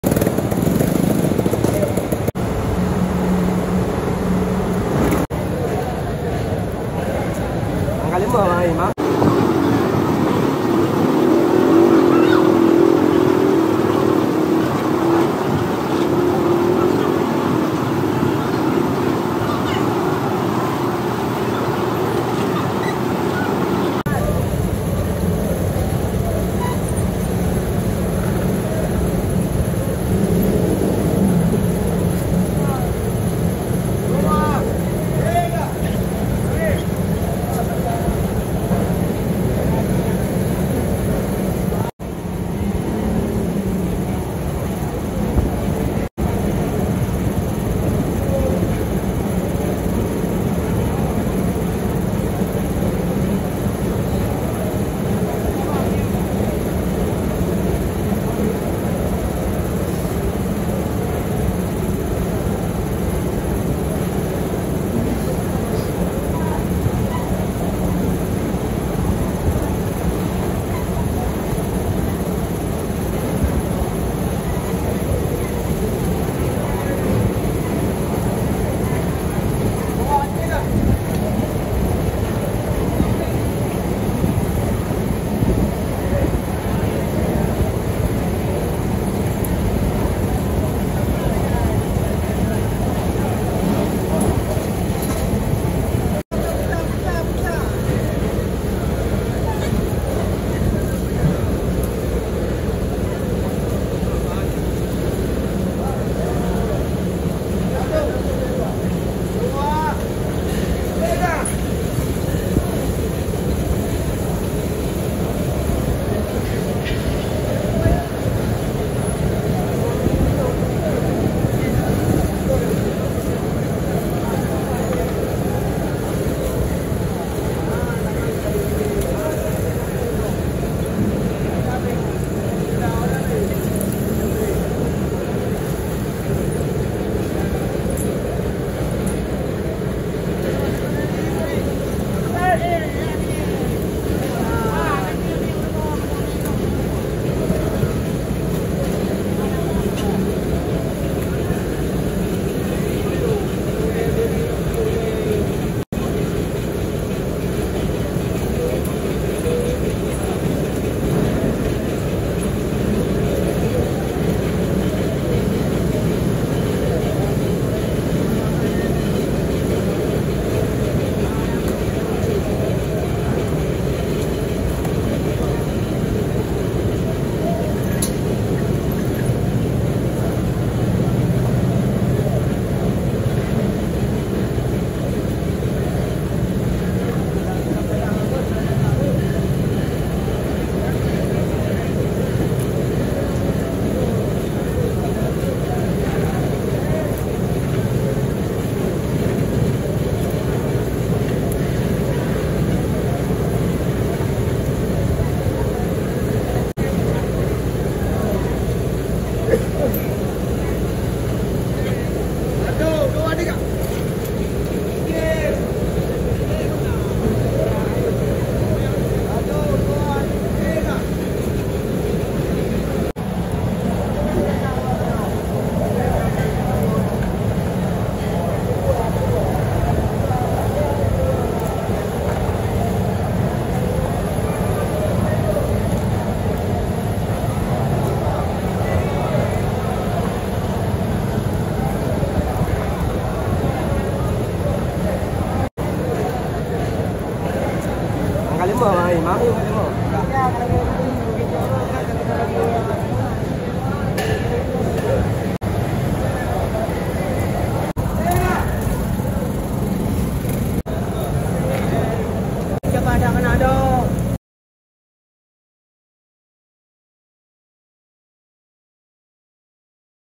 How Asia fishermen catch billions of baby fish unloading in traditional market - The fastest unload
Life on board a commercial fishing vessel in a traditional market.

Experience life aboard a commercial fishing vessel on an immersive journey into the daily lives of its crew. From high seas challenges to shipboard camaraderie, discover the fascinating world of deep sea fishing.